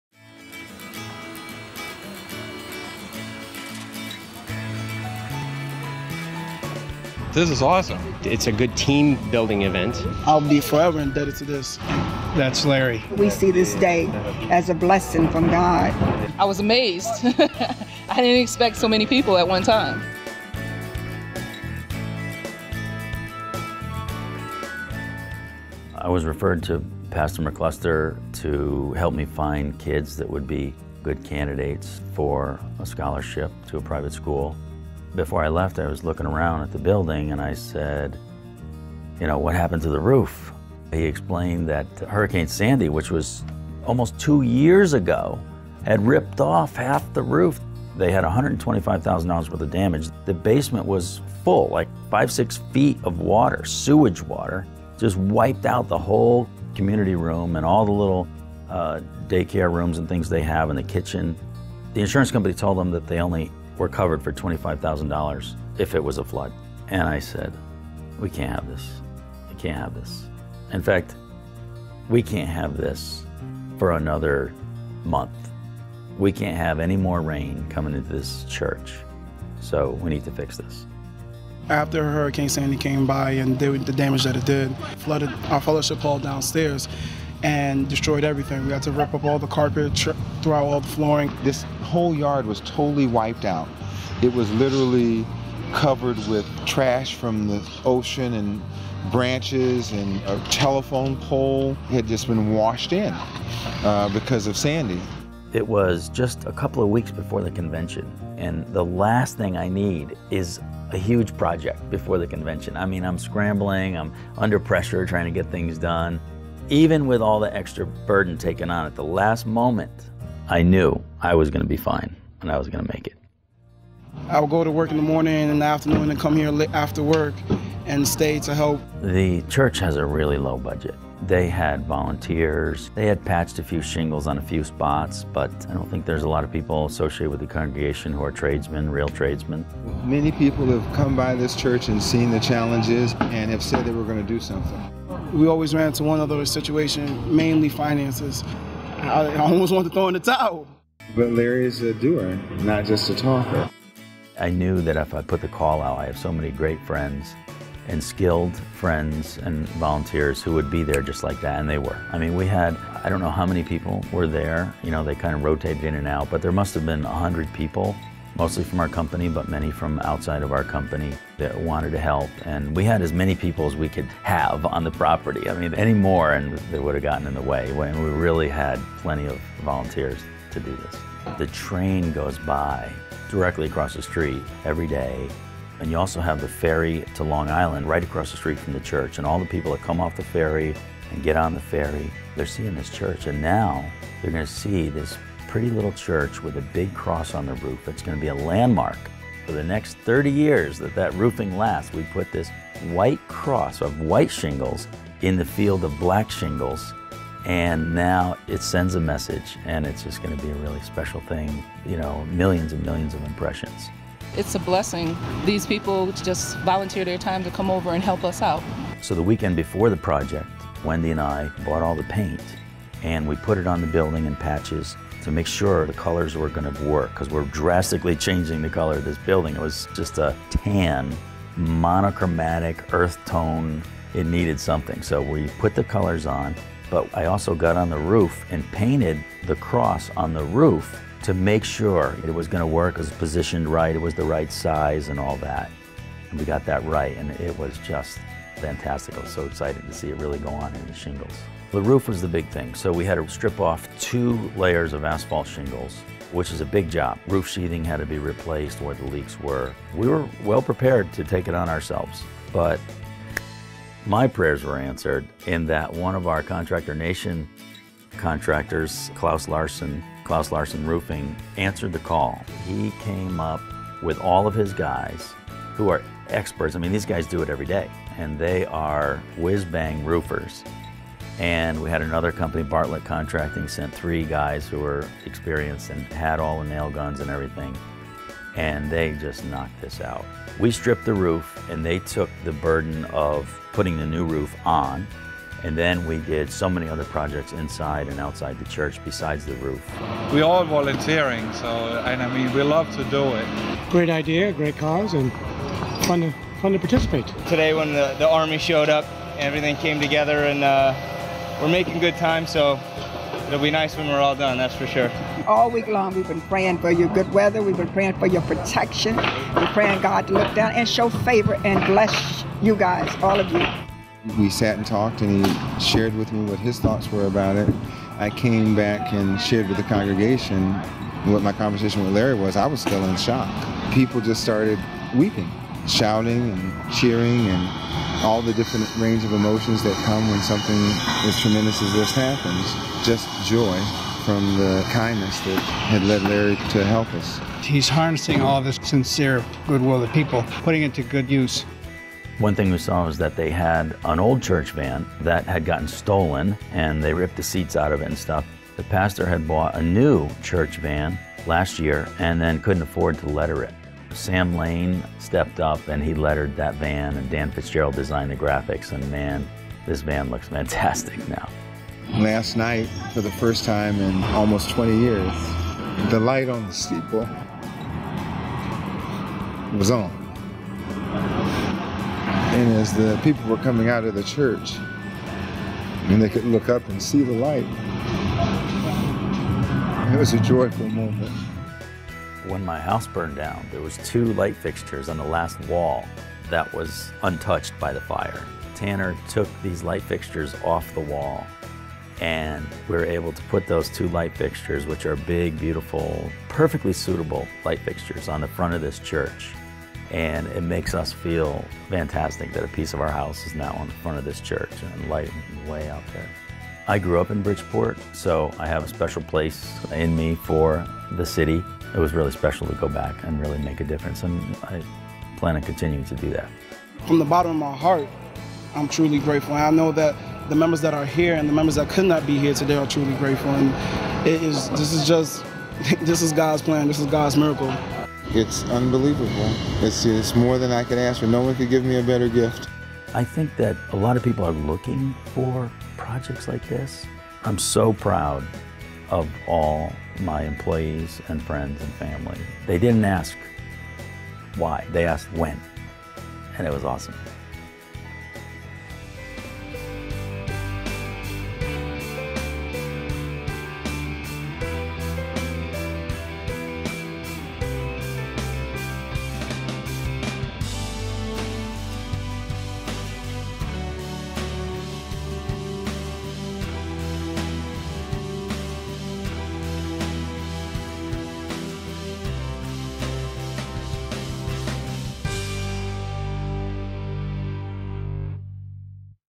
Team Basement Systems Helps Restore Hurricane-Damaged Shiloh Baptist Church in Bridgeport, CT
As supporter of a local scholarship program, Larry Janesky, owner and founder of Basement Systems, Inc., was actively looking for possible recipients when he was referred to Rev. Carl McCluster, the senior pastor at Shiloh Baptist Church, for help with finding applicants.

During his meeting with the pastor, Larry had the opportunity to walk around the church and noticed that the roof of the building was badly damaged. He asked Rev. McCluster what had caused all the damage and the pastor explained that half of the roof was destroyed by Hurricane Sandy in October 2012.

That devastating storm not only damaged the roof of the Shiloh Baptist Church, but also flooded the basement with 5 to 6 feet of sewage water, completely ruining the day care center, the community room and the kitchen, which were all located in the flooded area.

The building suffered $125,000 worth of damages, but the insurance company would only cover $25,000, leaving the congregation to pay for the bulk of the repairs. Shiloh Baptist Church operates on a tight budget, spent mostly on programs to help the local community. For that reason, since the fall of 2012, they have been struggling to obtain both funds and professional work to get the building properly restored.

Members of the congregation had helped with cleaning up and patching some shingles on the roof, but while they did their best, there weren't any real tradesmen in the congregation to professionally install a much needed brand new roof for the Bridgeport church.

When Larry learned this, he realized that this small, but caring congregation had suffered long enough. After almost 2 years of dealing with water leaking through the roof every time it rained, they couldn't be made to suffer through it any longer, so Larry decided to step in and help out.

Most importantly, he decided to do it right away. Larry knew he had some employees and some good, skilled friends in the vicinity that would be ready to volunteer for a project like this.
Larry and his team at Basement Systems were all very busy at the time of renovations, as they had also been preparing for the company’s annual convention – a national 3-day event that would bring 1,200 people from all over the U.S. and Canada to the state.

Yet, even pressed by time, many employees volunteered and many of Larry’s friends stepped in to provide their services. There were approximately 100 people involved in the project, arriving in rotating shifts. “I am so proud of my employees, friends and family”, says Larry. “They didn't ask me why. They asked when. And that was awesome!”

The roof was the biggest challenge. There were two layers of asphalt shingles to be taken off and while Larry and his team were well prepared to take on the project on their own, Klaus Larsen from Klaus Larsen Roofing answered and took over that part of the project bringing in highly skilled volunteers. Bartlett Contractors also pitched in sending professionals in their network to tackle the project.

Besides the roof, many other repairs were performed both inside and outside the building – which also received brand new interior and exterior painting jobs. Even the church’s new van received new lettering!

The church’s exterior is now painted in beautiful light blue shades and white trims, and a cross of white shingles designed on the brand new roof now gleams under the sun, and can be seen by the passengers from the train cars that run next to the church, and by the people coming and going through the Long Island ferry, just across the street.

Larry and the team at Basement Systems would like to thank the entire Shiloh Baptist Church congregation for the warm welcome, the prayers, the help and the opportunity to give something back to the community. We also would like to thank all of our volunteers, with special thanks to Dr. Energy Saver of CT, Klaus Larsen Roofing, Bartlett Contractors, and CT Electrical Services, ABC Supply & Co. for all their help and support.